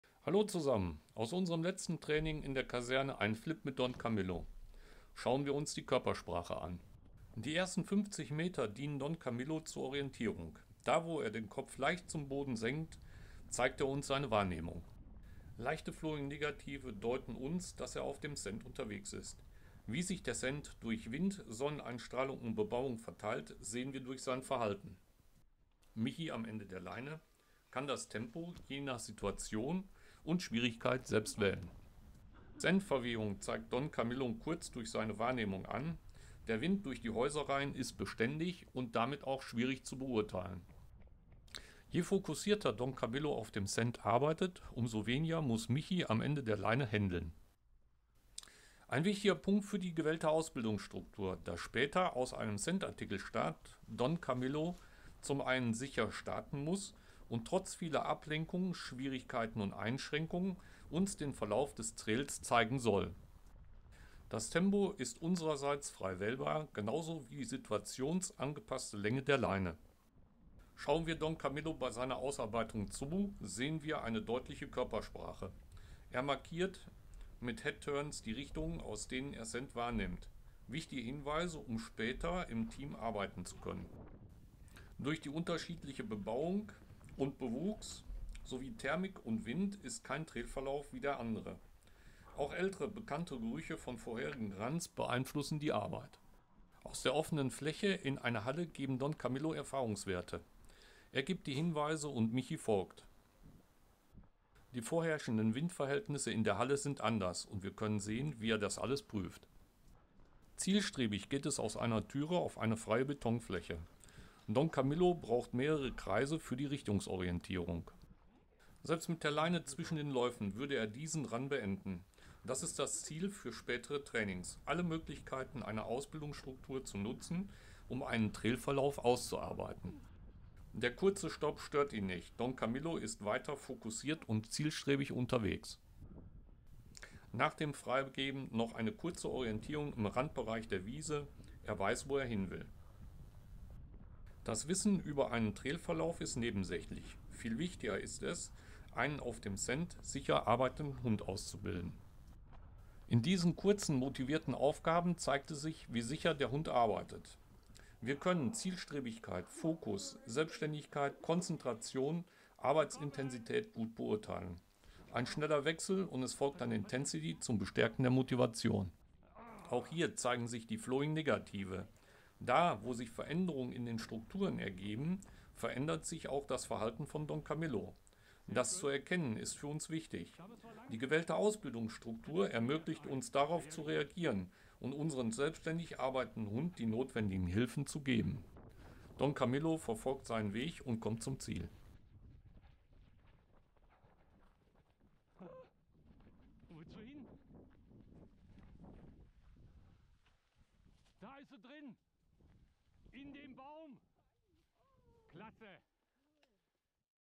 Mantrailing - Wie der Hund mit uns kommuniziert 6 - Die Kocher Methode
Die wichtigsten Fragen: Hat er einen Trail oder hat er keinen? Was erkenne ich an meinem Hund?
Darauf zielt die Ausbildung ab. Je strukturierter, umso einfacher wird es. Es ist kein Ratespiel. Der Hund kann durch entsprechende Ausbildungstools geformt werden. Seine Körpersprache verbessert und verdeutlicht werden. Hier ein Beispiel eines Flips. Derjenige am Ende der Leine muss über den Verlauf nichts wissen. Es muss nichts abgesprochen werden. Viel wichtiger ist es, intuitiv am Ende der Leine handeln zu können.